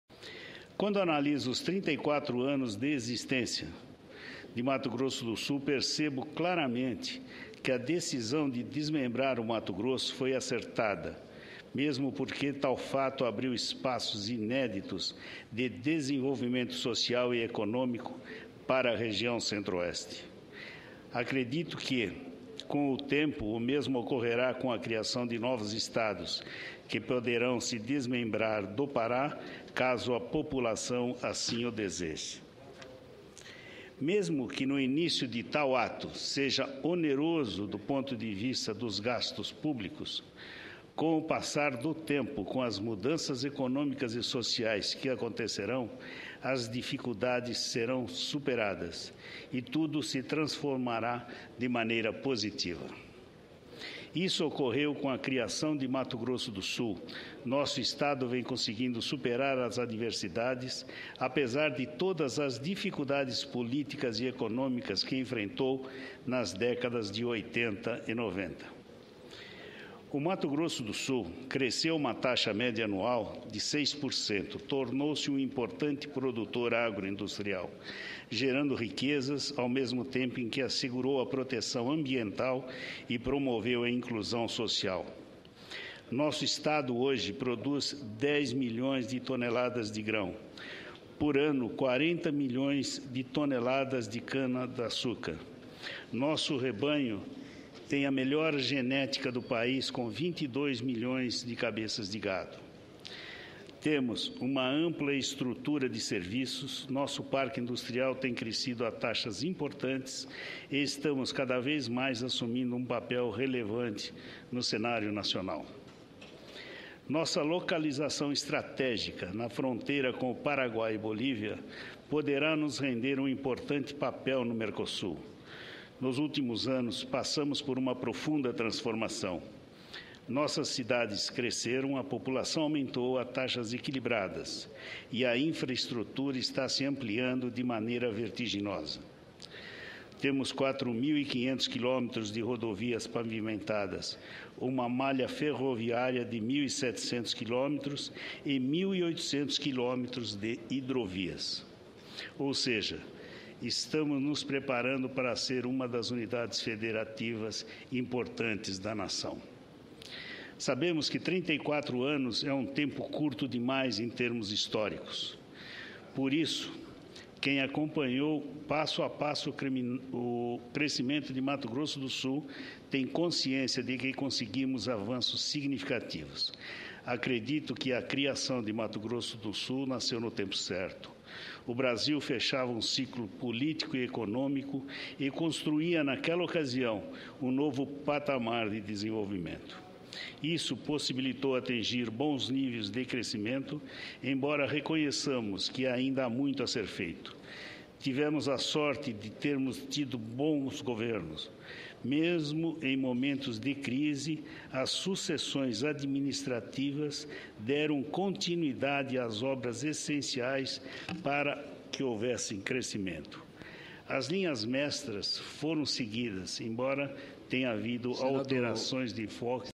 Desenvolvimento de Mato Grosso do Sul justifica divisão do estado, analisa sen. Antonio Russo
10/10/2011 - Plenário - Desenvolvimento de Mato Grosso do Sul justifica divisão do estado, analisa sen. Antonio Russo